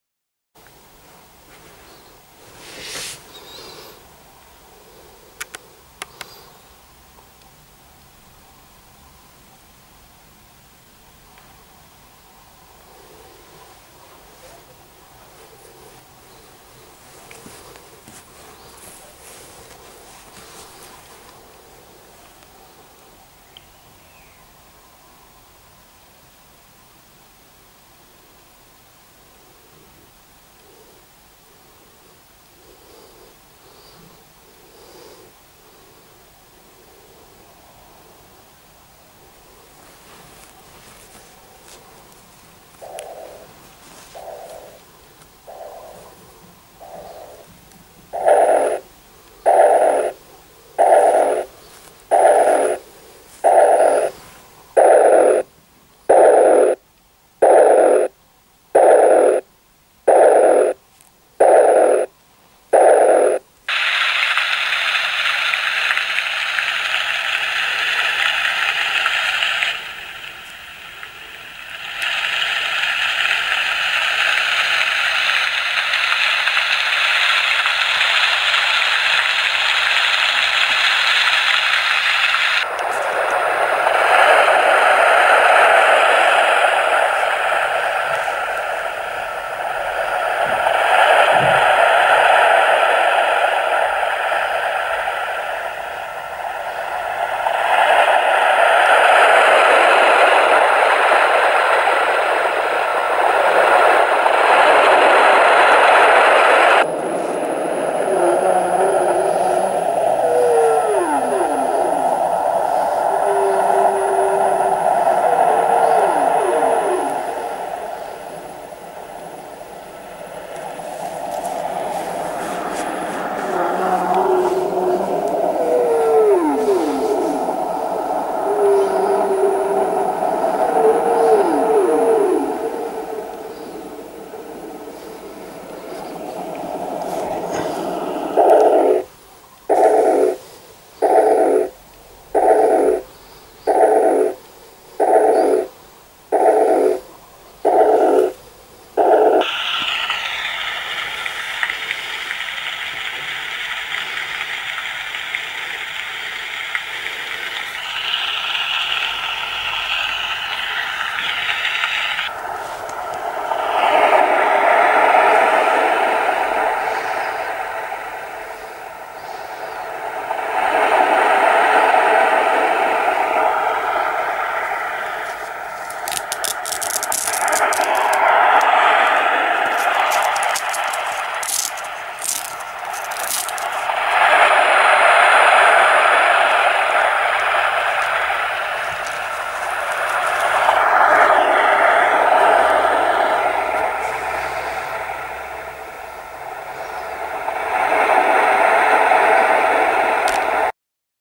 Sleep Sheep Cloud b Śpiąca Owieczka pomaga uśpić maluszka
Śpiąca  wieczka, przytulanka z pozytywką z czterema kojącymi dźwiękami doskonale pomagającymi zasnąć naszemu maluszkowi.Bicie serca matki, fale oceanu, wiosenny deszcz oraz piosenki wielorybów wprowadzą maluszka w spokojny sen.